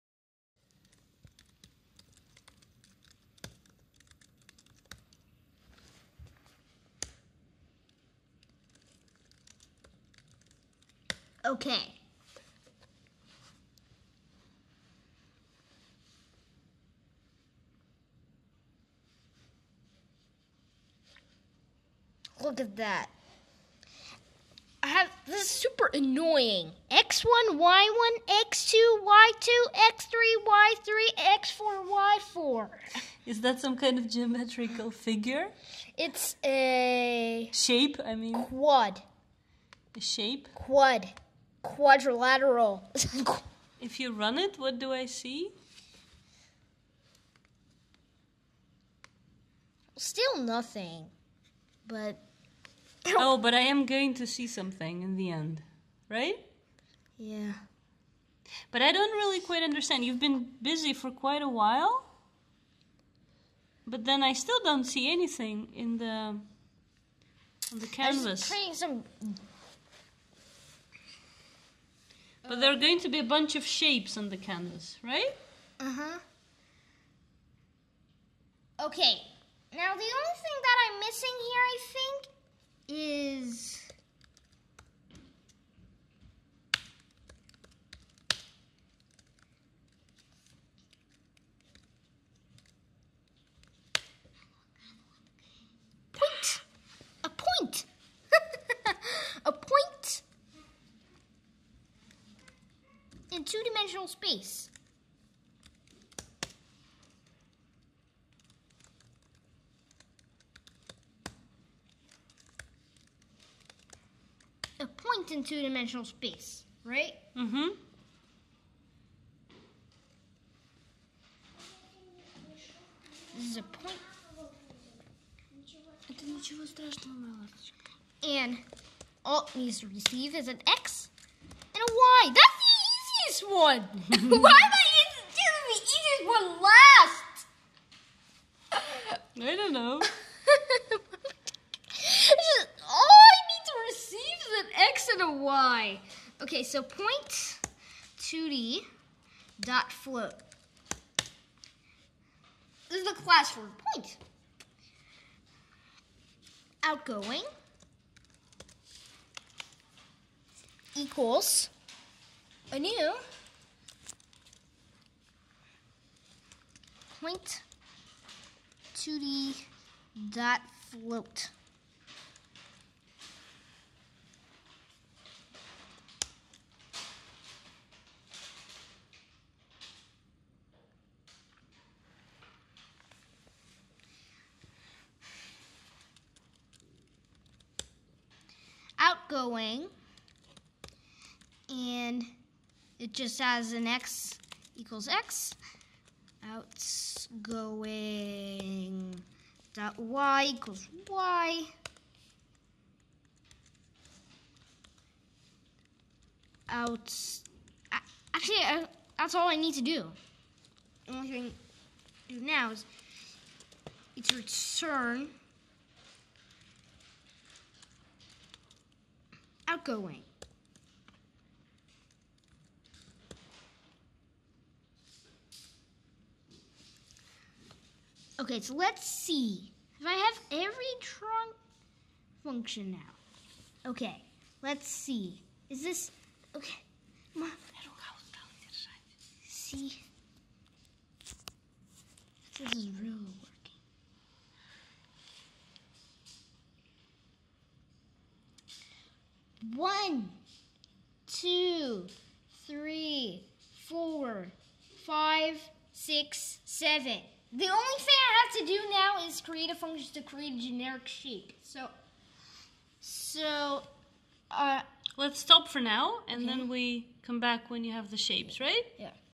Simon testing Codota 3
In the 4 videos in this set, Simon is building a Codota demo in Java. Codota is an AI programming assistant that is looking for solutions on GitHub and other global resources and suggests them in real time, recognizing your code. At the moment, it's only available for Java and only for three editors (here - Eclipse), so the use is very limited, but their website says that other languages will follow soon. Since Simon normally uses Processing for Java, he can't really use Codota for most of his projects. It has been an interesting exercise though (and I was surprised at how skillful he is at writing Java in Eclipse, which is quite different from Processing), and a glimpse into the future. There's no doubt assistants such as Codota will very soon become a common companion. Simon had Codota resolve one error for him and was very happy about that. He said Codota was his friend. He was reluctant to turn its speech functions on, however. Simon has this slight fear of full blown AI and a fascination, wanting to learn how it works, at the same time.